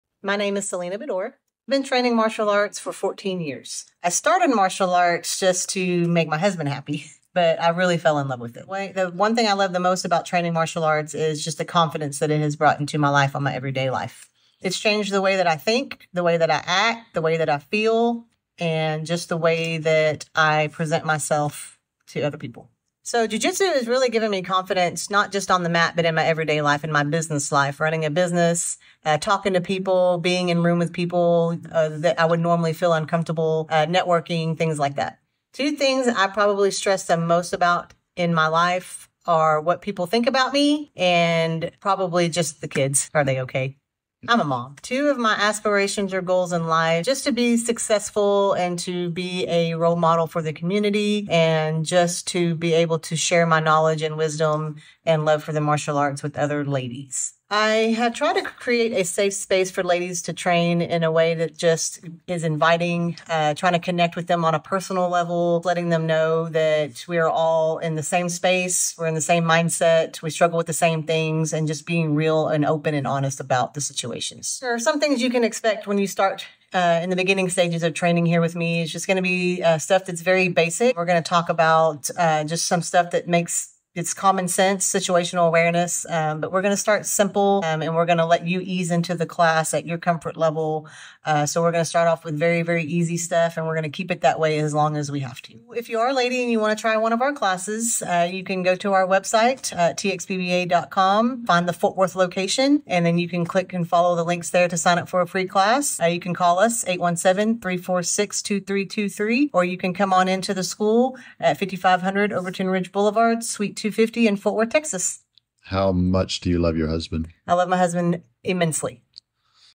Interview with a BJJ instructor.
Meet Professor Celeena Bedore. She loves empowering women using Jiu Jitsu and self defense.
To find out more visit ladiesbjj.txbba.com